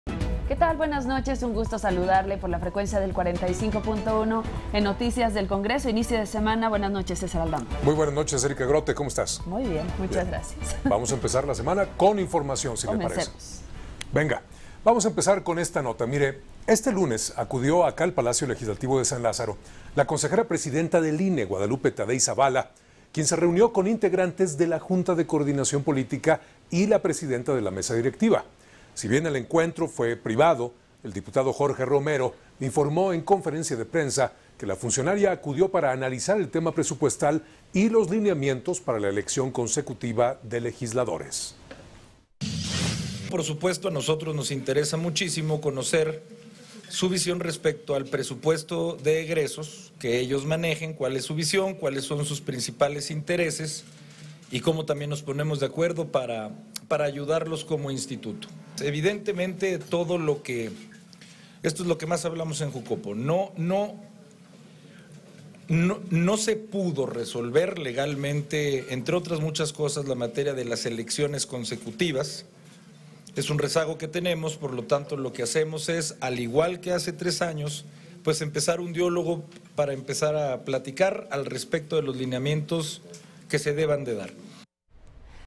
Se reúne Jucopo con presidenta del INE
Acudió al Palacio Legislativo de San Lázaro la consejera presidenta del INE Guadalupe Taddei Zavala, quien se reunió con integrantes de la Junta de Coordinación Política y la presidenta de la Mesa Directiva. Si bien el encentro fue privado, el diputado Jorge Romero informó, en conferencia de prensa, que la funcionaria acudió para analizar el tema presupuestal y los lineamientos para la elección consecutiva de legisladores.